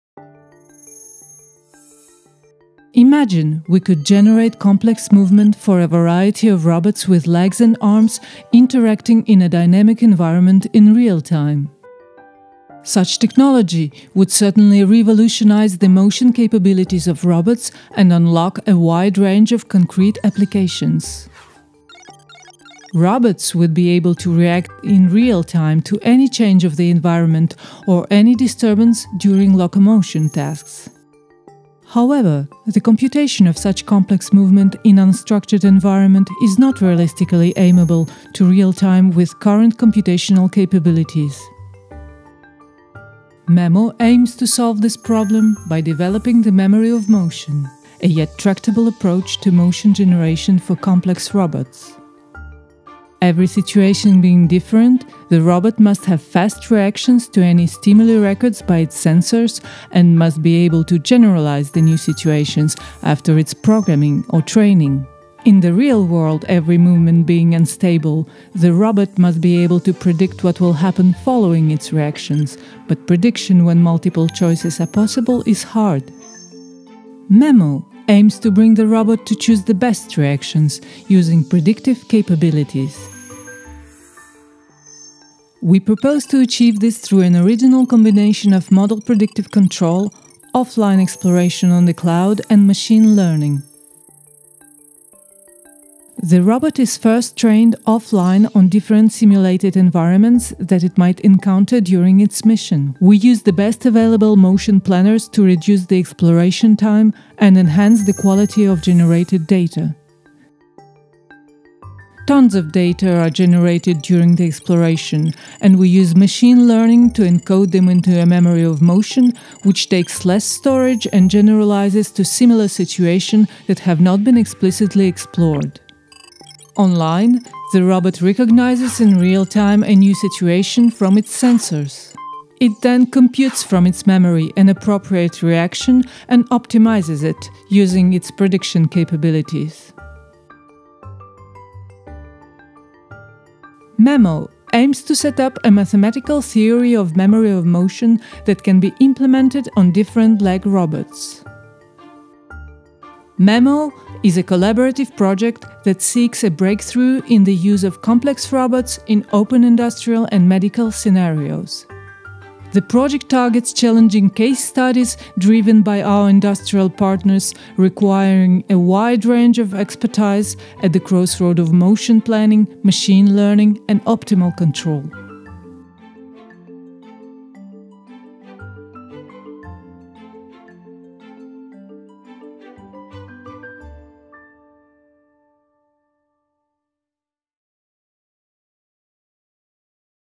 Memory of Motion: motion learning for real robots with arms and legs
What if we could generate complex movements for arbitrary robots with arms and legs interacting in a dynamic environment in real-time? Such a technology would certainly revolutionize the motion capabilities of robots and unlock a wide range of very concrete industrial and service applications: robots would be able to react in real-time to any change of the environment or unexpected disturbance during locomotion or manipulation tasks. However, the computation of complex movements for robots with arms and legs in multi-contact scenarios in unstructured environments is not realistically amenable to real-time with current computational capabilities and numerical algorithms.

The project Memmo aims to solve this problem by 1) relying on massive off-line caching of pre-computed optimal motions that are 2) recovered and adapted online to new situations with real-time tractable model predictive control and where 3) all available sensor modalities are exploited for feedback control going beyond the mere state of the robot for more robust behaviors. Memmo will develop a unified yet tractable approach to motion generation for complex robots with arms and legs.